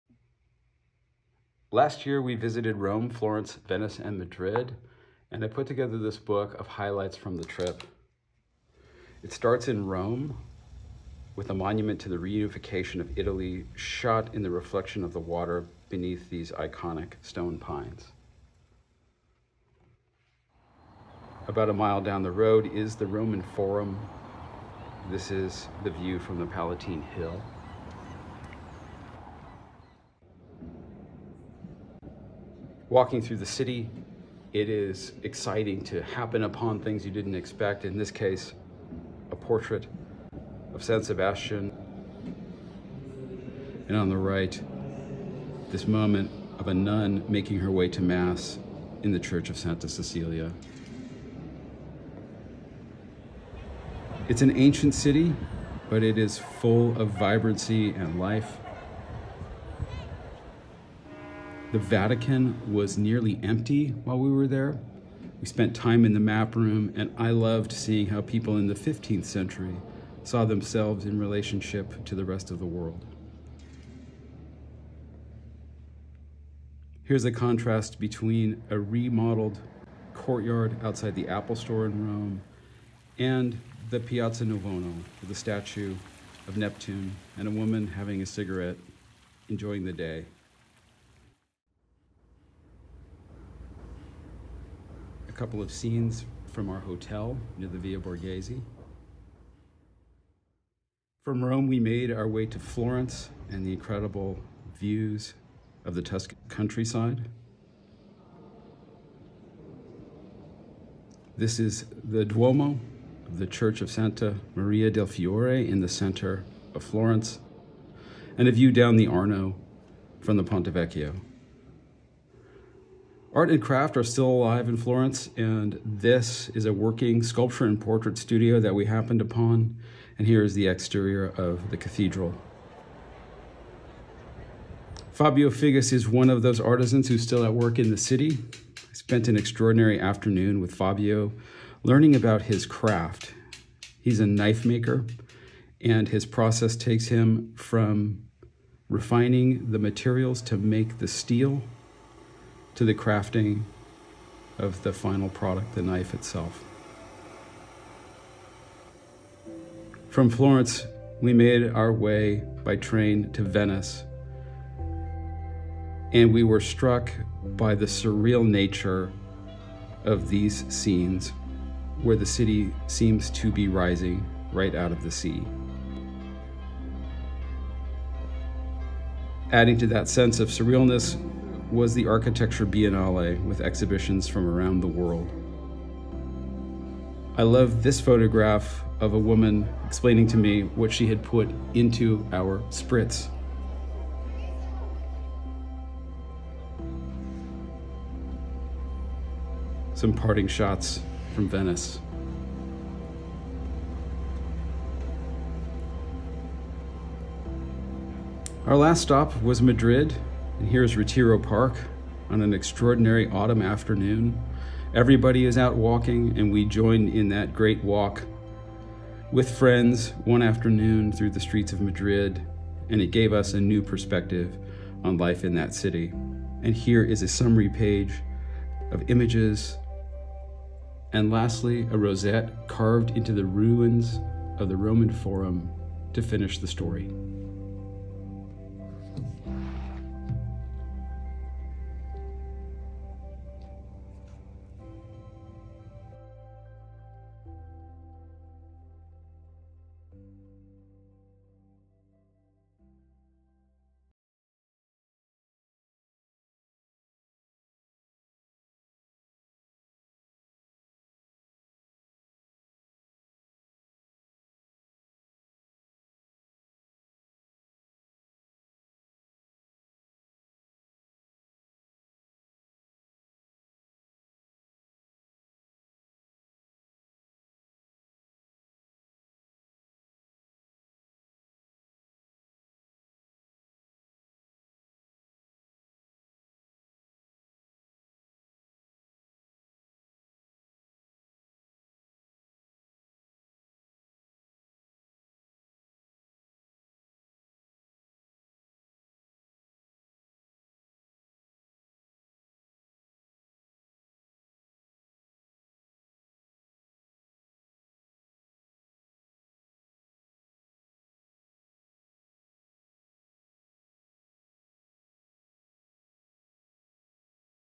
Rome, Florence, Venice, Madrid
This book captures the highlights of our visit. The still images are complemented by ambient sounds captured on the day.

Nearly all of the images were shot on the iPhone.